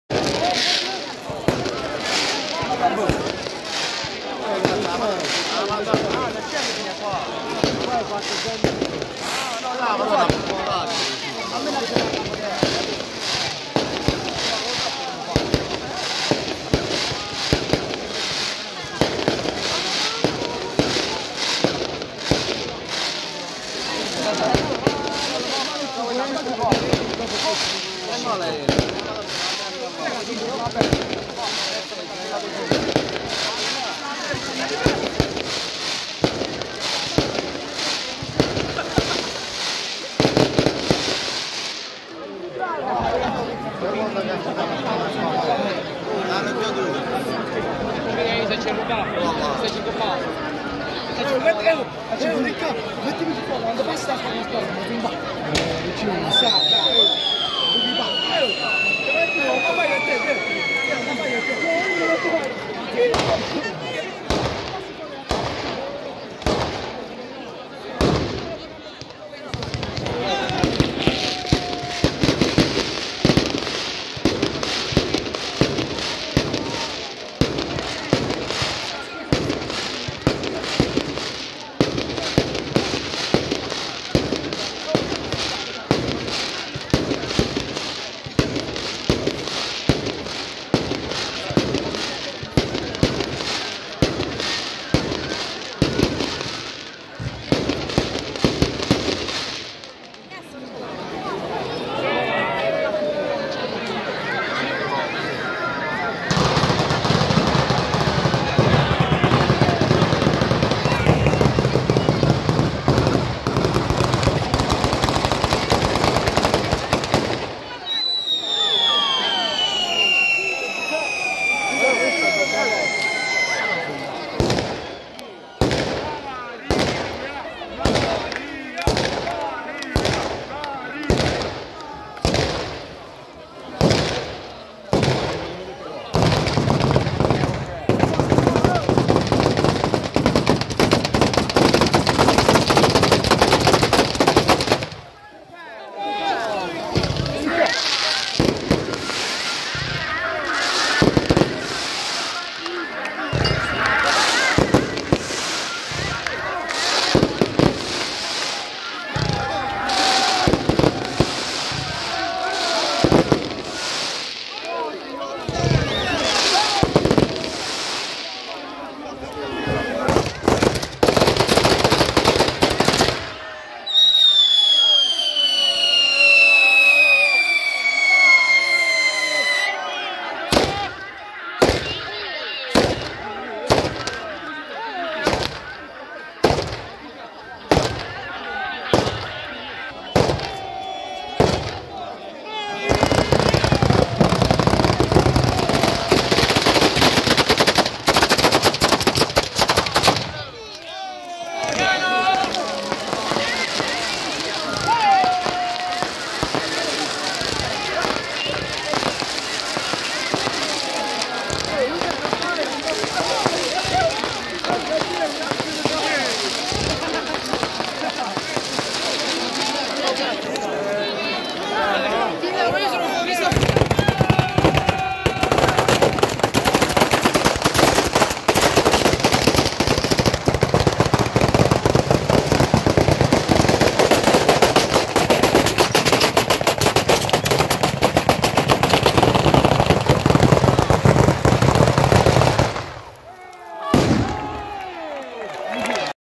Apricena (Fg) Solenni Festeggiamenti in onore di Maria SS. Incoronata 2024 Batteria processionale
Apricena (Fg) Solenni Festeggiamenti in onore di Maria Santissima Incoronata 2024
Batteria pirotecnica durante la processione